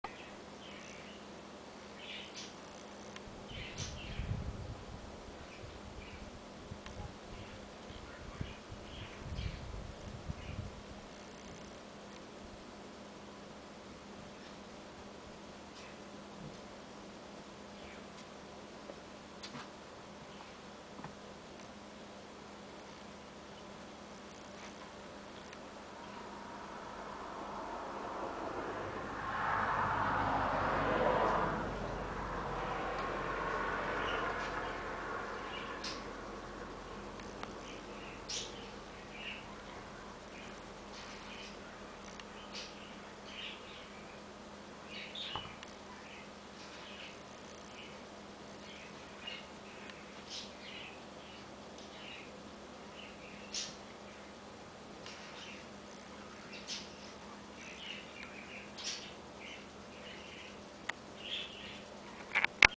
my visiting yuma bat
perching, trembling
my yuma: she sits watching
and how long till dusk?